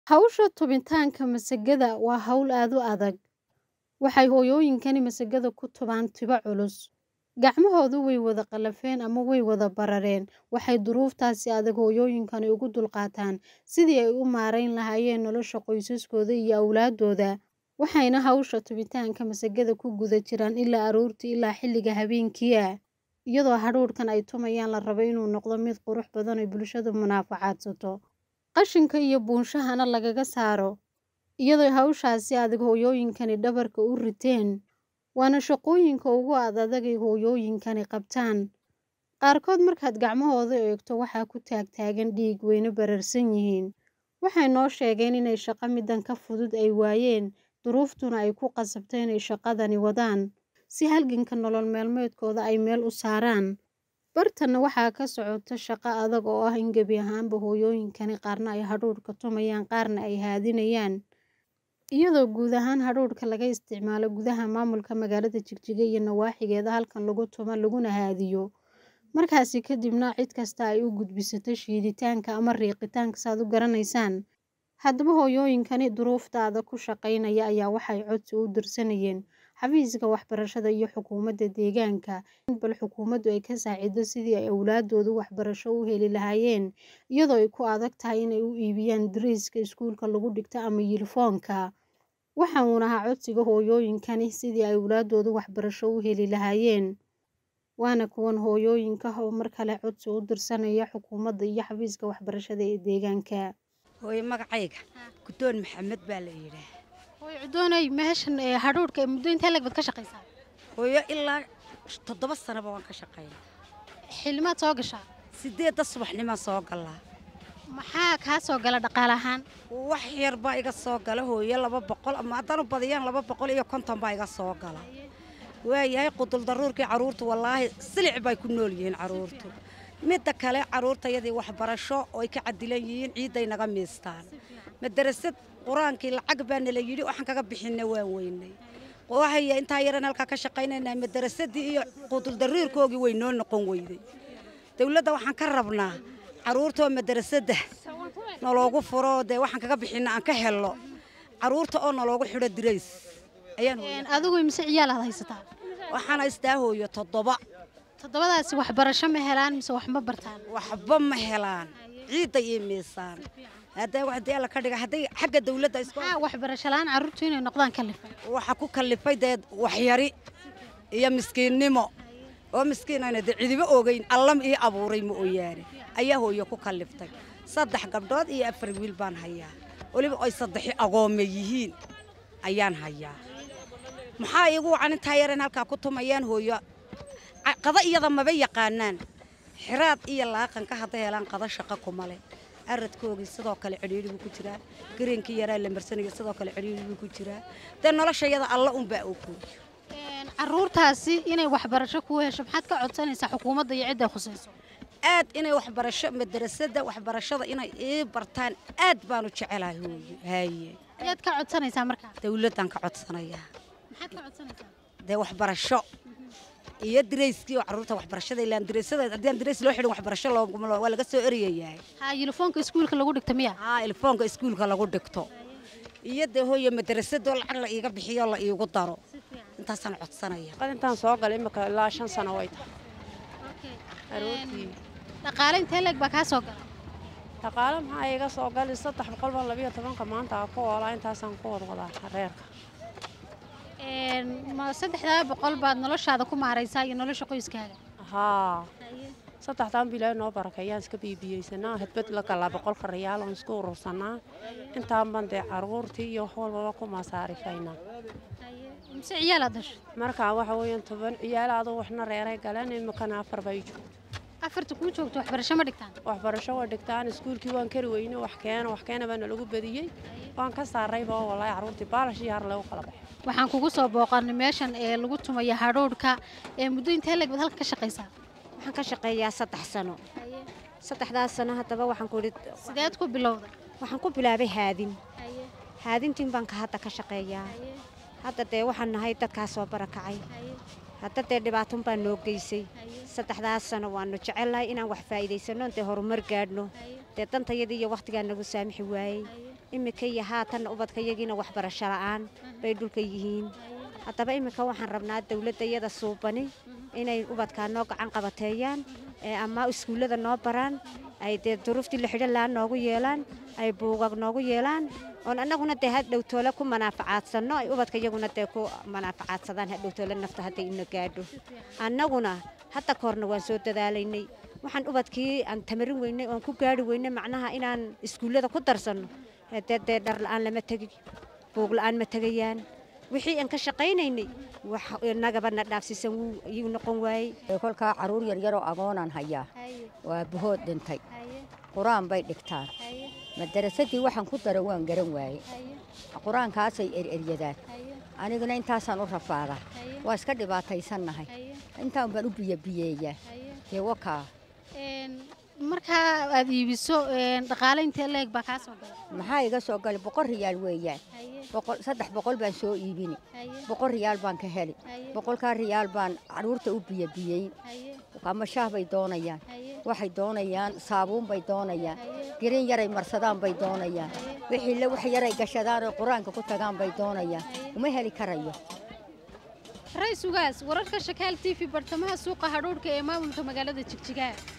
Warbixin:-qaar kamida hooyooyinka ka ganacsada tumista iyo haadinta hadhuudhka.
@ShakaalTv ku xayaasiiso ganacasigaaga
@ShakaalTv  kahel warar iyo warbixino lahubo
@ShakaalTv waheelka bulshada somaliyeed